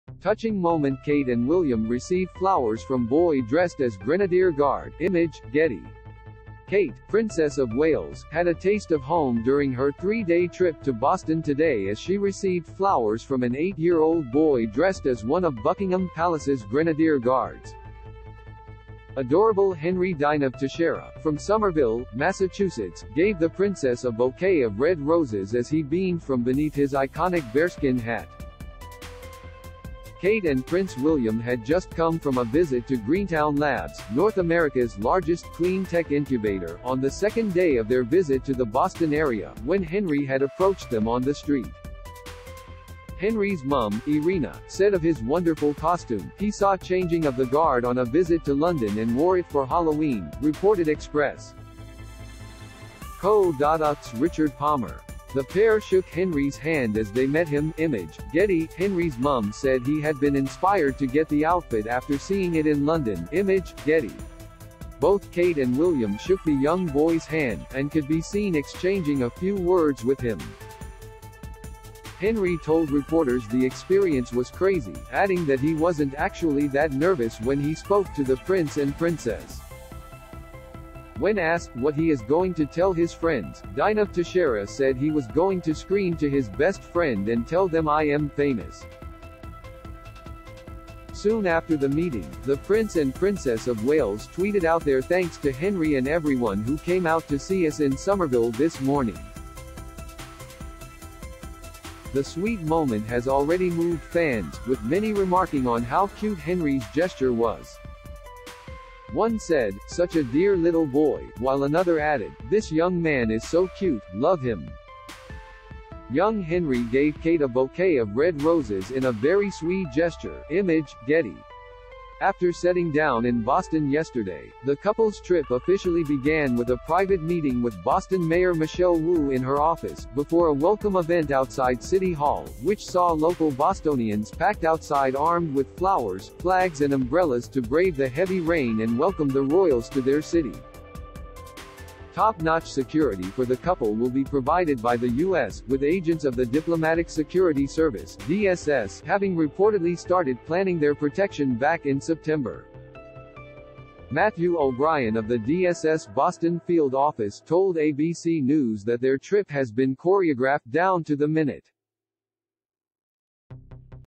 Kate and William receive flowers from boy dressed as Grenadier Guard
Touching moment Kate and William receive flowers from boy dressed as Grenadier Guard (Image: Getty) Kate, Princess of Wales, had a taste of home during her three-day trip to Boston today as she received flowers from an eight-year-old boy dressed as one of Buckingham Palace's Grenadier Guards.  Adorable Henry Dynov-Teixeira, from Somerville, Massachusetts, gave the Princess a bouquet of red roses as he beamed from beneath his iconic bearskin hat. Kate and Prince William had just come from a visit